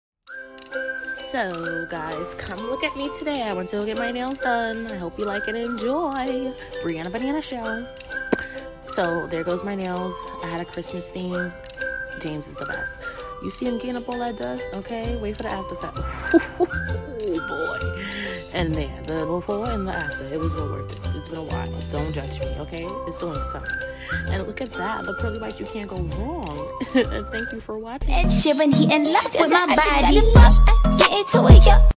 Day with me // My First Mini Vlog + Nail Day
Get a peak of what’s in store starting February 1st, 2022 I’ll be posting my first real vlog and I’m excited for you guys to see. I’m going to post several vlogs for you guys to get a feel of my channel and than weekly from then on. I hope you all enjoy this video and keep coming back for more. 🥰